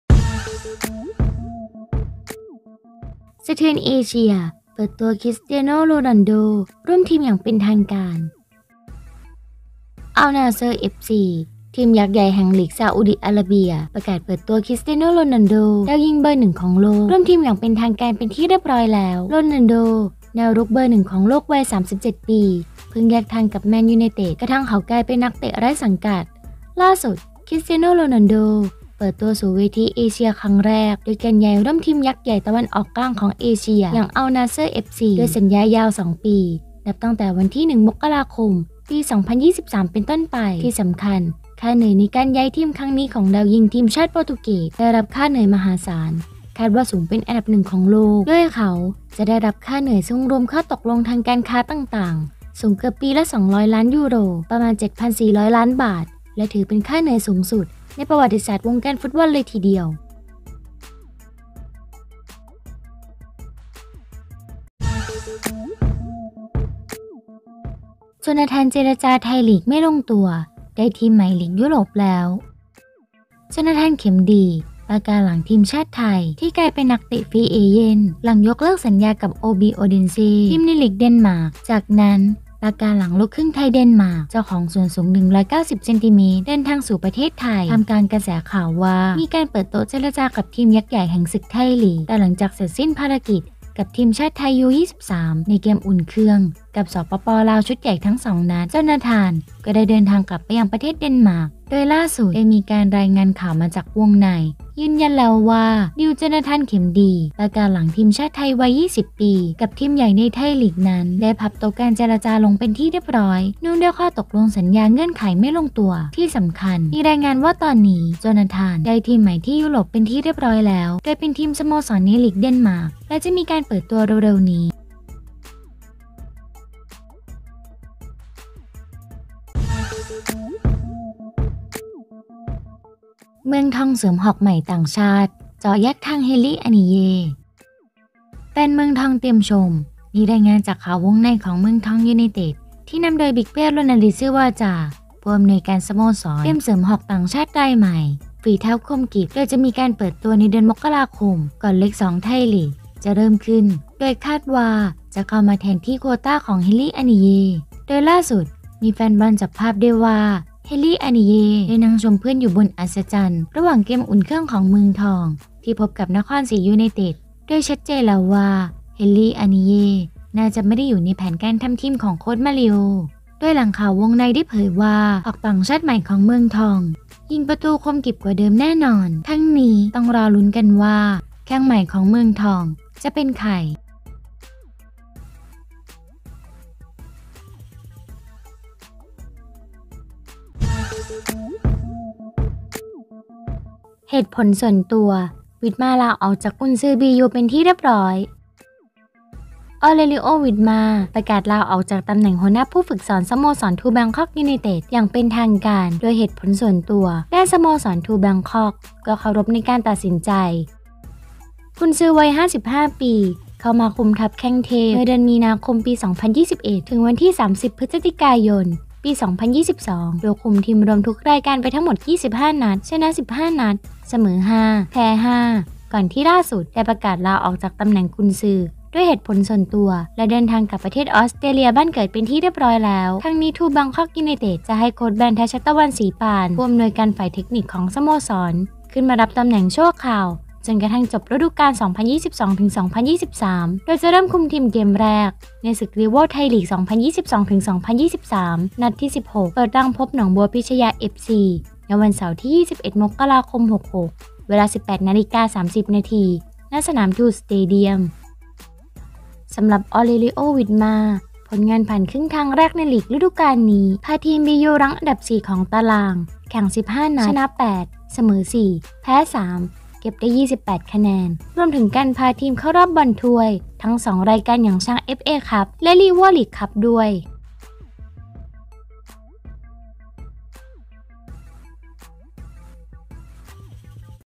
สะเทือนเอเชีย เปิดตัว "คริสเตียโน่ โรนัลโด้" ร่วมทีมอย่างเป็นทางการ
Al-Nasar FC ทีมยักษ์ใหญ่แห่งลีก ซาอุดิอาระเบีย

ประกาศเปิดตัว "คริสเตียโน่ โรนัลโด้" ดาวยิงเบอร์หนึ่งของโลก ร่วมทีมอย่างเป็นทางการเป็นที่เรียบร้อย